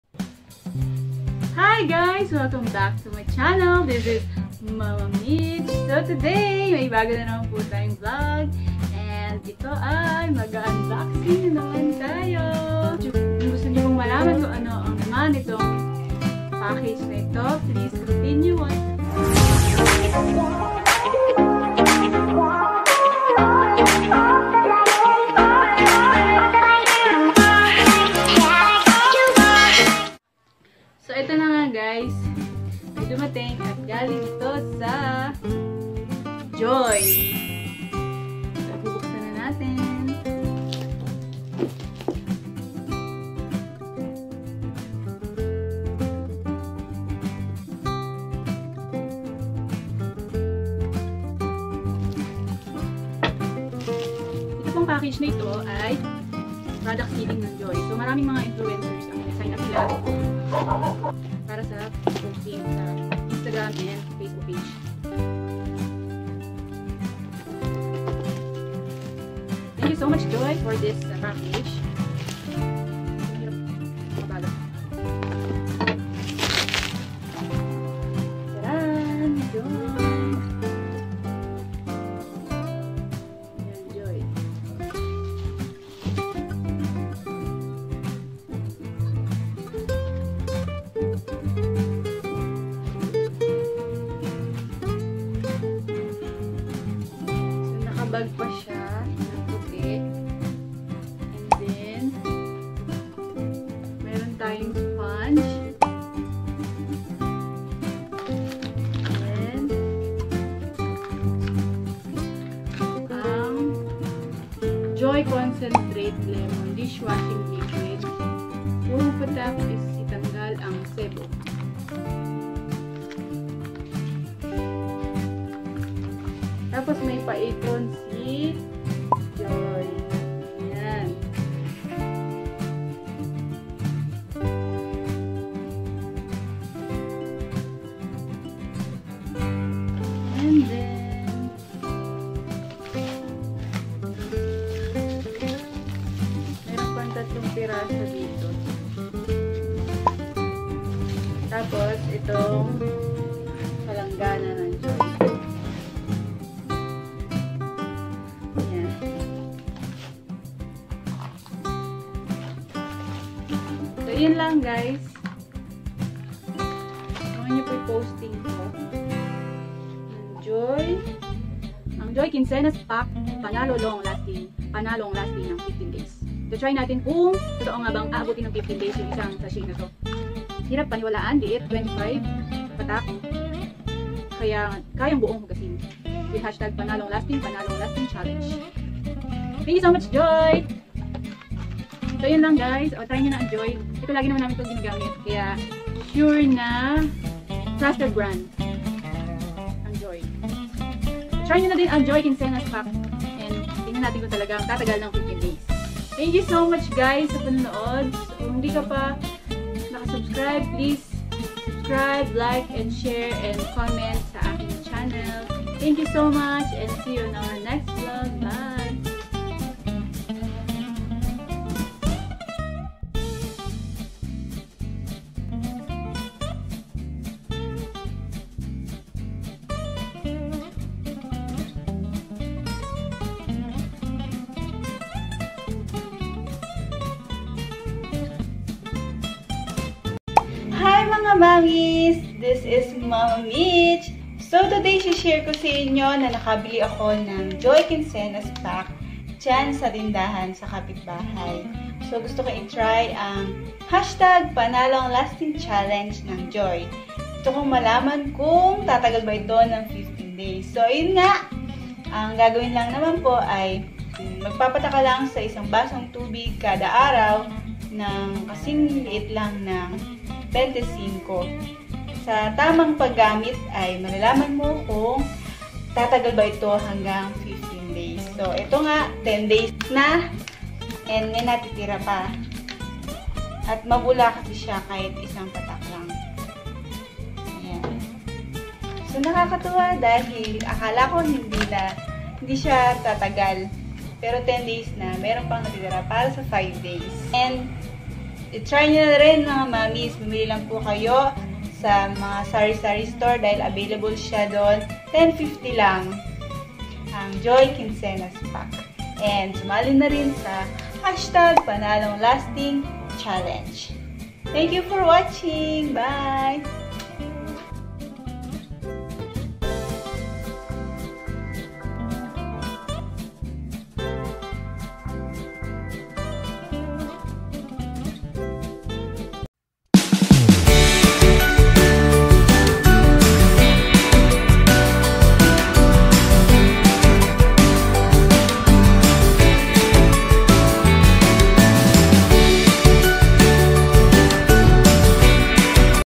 UNBOXING: JOY KINSENAS PACK! Aabot ng 15days| MichelleLalic
Hi mga Ma! Isa talaga sa hindi maubos na gawain sa bahay ang paghuhugas ng pinagkainan, di ba? Ayokong natatambakan ng hugasin kaya lagi kaming may joy, kapartner nating mga inay sa malinis na gamit sa kusina!

Tara at itry natin ang Joy Kinsenas Pack kung tatagal ba hanggang 15 days. Sali na sa PanalongLastingChallenge ng Joy.

Eto ang dapat gawin: Magpatak lamang po ng halos kasing liit ng bente singko ng joy dishwashing liquid sa isang basong tubig kada araw para malaman natin kung abot ng 15days.

Mabibili ito sa mga sari-sari stores, kaya wag na magpahuli at sali na sa challenge.

MommyLevyTribe Joy Philippines Mommy Levy